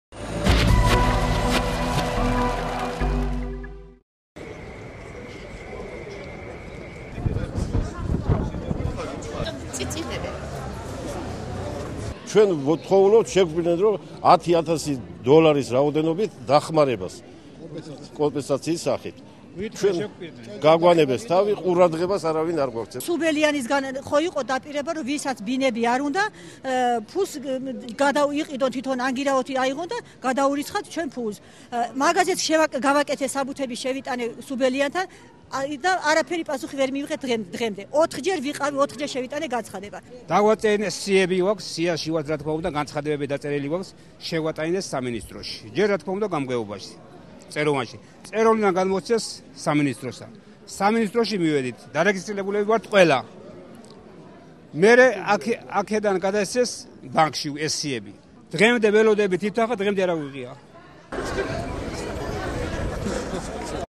05/02/2010
ახალგორიდან დევნილმა მოსახლეობამ კომპენსაციების მოთხოვნით პარლამენტთან აქცია გამართა